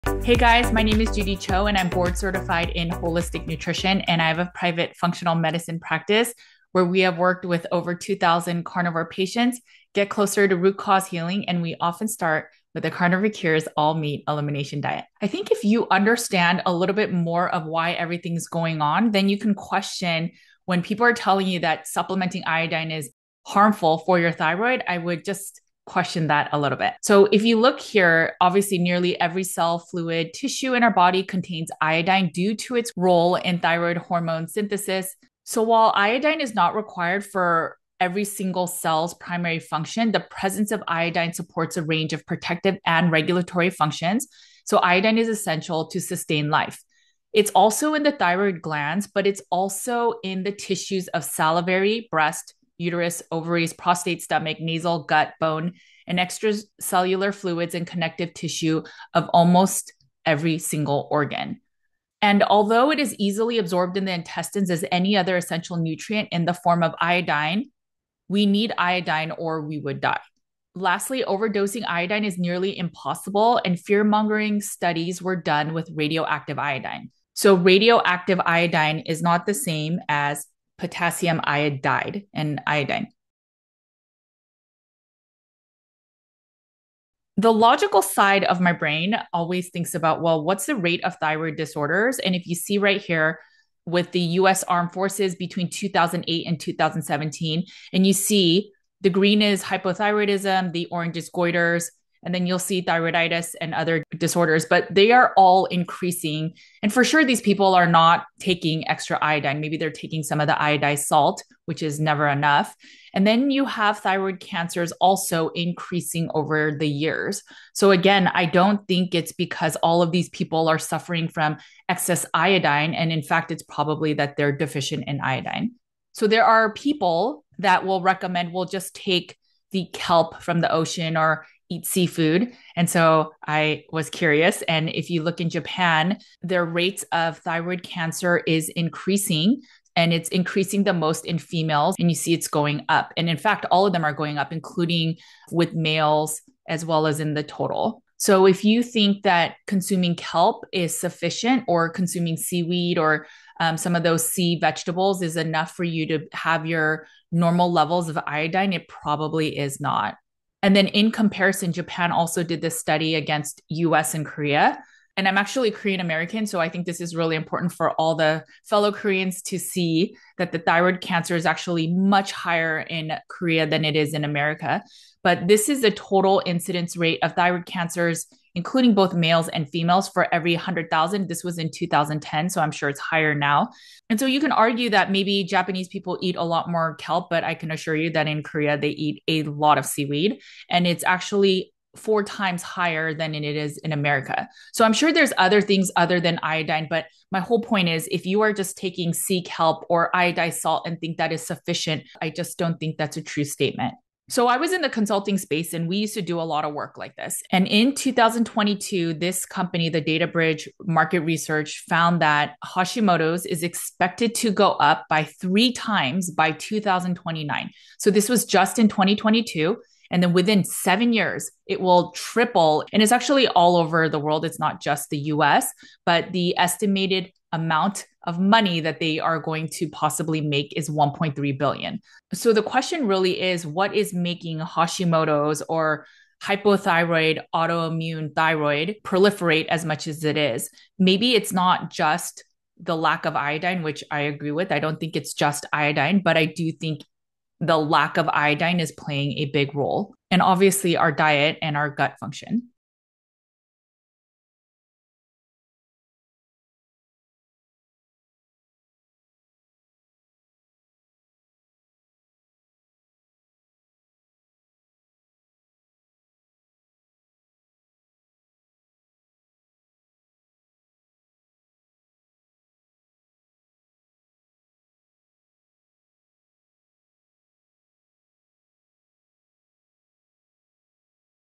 JUDY CHO i1 | IODINE DEFICIENCY …main driver of HASHIMOTOS MANY THYROID DISEASES
presents episode 2367 | JUDY CHO of NutritionwithJudy.com

Iodine
nearly every cell, fluid, tissue contains iodine, due to:
-hormone synthesis; antioxidant properties; evolutionary importance
-essential for regulatory functions

fills tissue of thyroid, salivary glands, breast, uterus, ovaries, prostate,
stomach, nasal, gut, bone, extracellular fluids

as important to life as vitamin D

nearly impossible to overdose iodine supplementing
-this is Not radioactive iodine--as was thought in 1950s

Study, Thyroid disorder growth, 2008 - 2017, military persons
not taking iodine supplements: up 5 to 27%
Also, thyroid cancers increasing every year 2000 to 2008
Eat more kelp and seafood, like the Japanese…
Their rates of thyroid cancers is increasing, the most in females,
up 13%
Korea, another kelp eating
area: thyroid cancer 4X
higher than in US

Hashimotos disease...research in 2022: by 2029 rise by 3X…
-worldwide, Why?
Judy Cho: iodine deficiency is playing a big role, along with diet